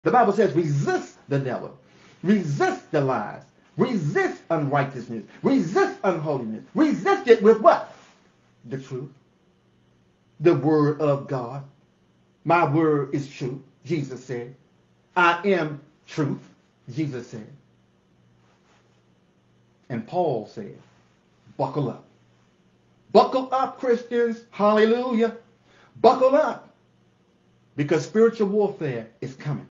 Prepare for Spiritual Warfare: Resist the Devil's Lies
We confront the powerful message from the Bible about resisting unrighteousness and embracing the truth of God's word. Join us as we equip ourselves for spiritual battles with faith and resilience. Buckle up, Christians! Hallelujah!